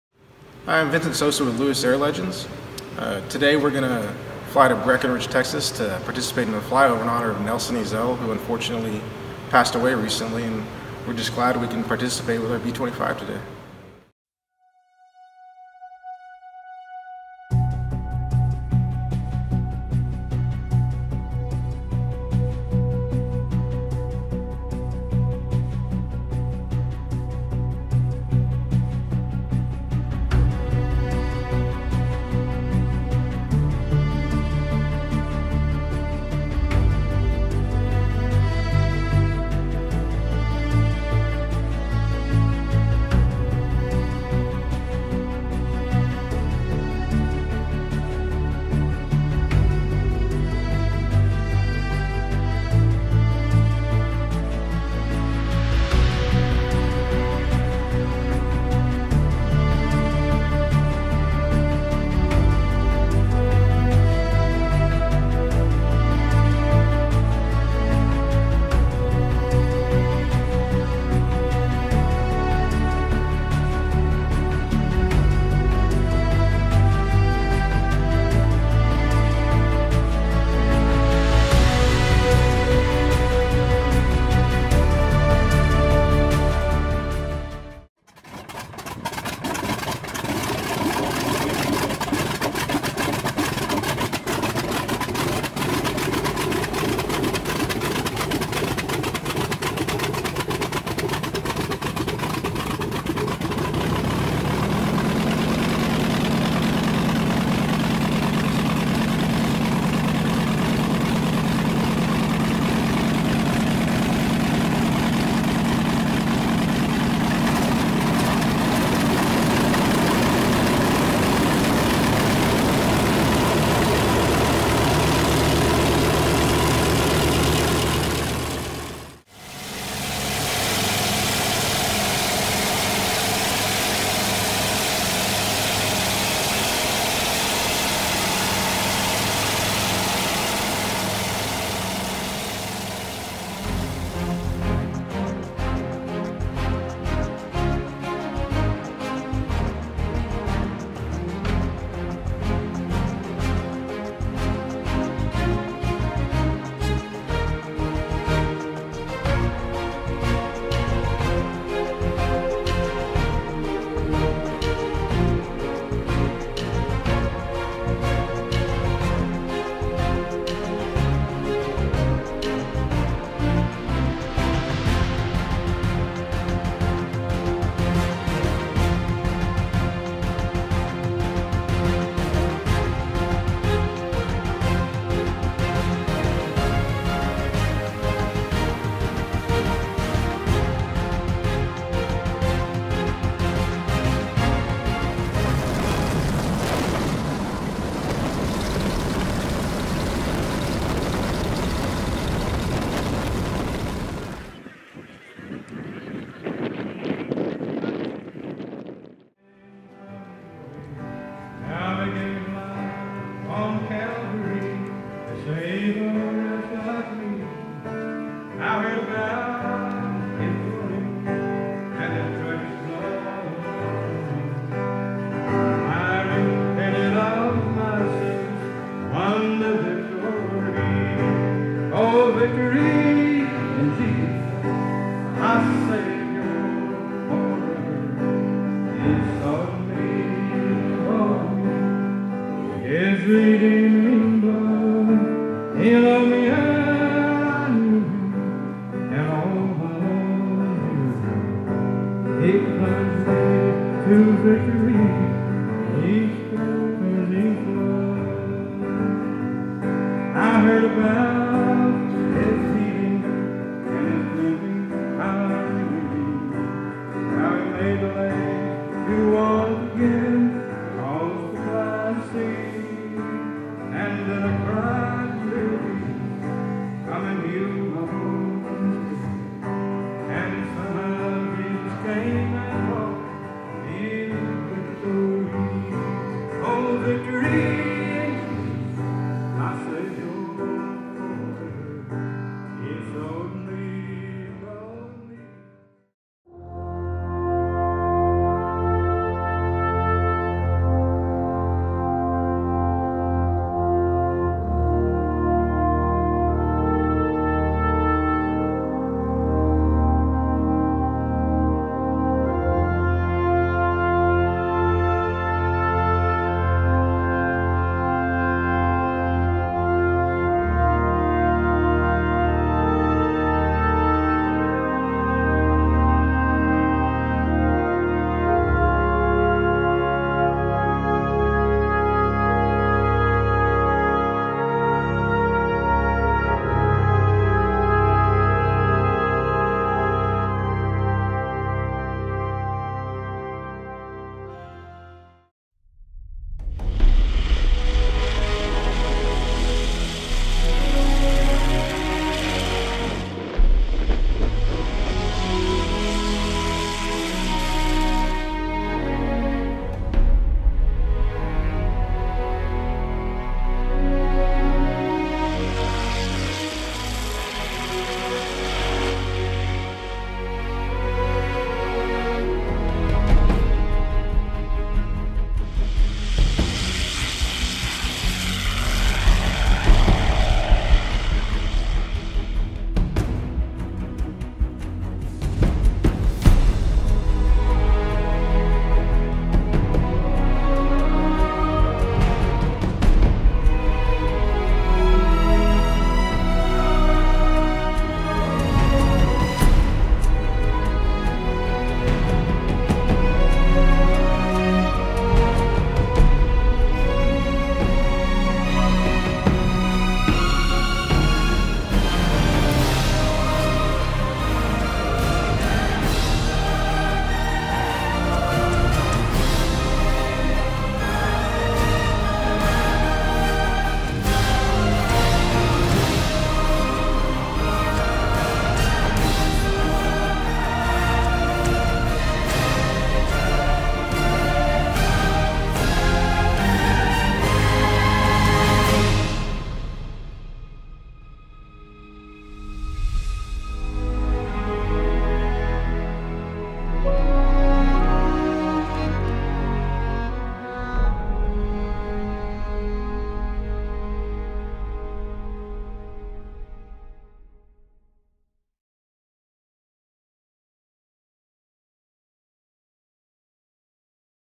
Nelson Ezell Missing Man Fly Over
Nelson Ezell was and continues to be an inspiration within the aviation community. He and his team restored thirty-four aircraft and performed repairs on over 100 different types of vintage airplanes- including some of Lewis Air Legends very own.

His legacy will continue to live on. This flight was to honor him.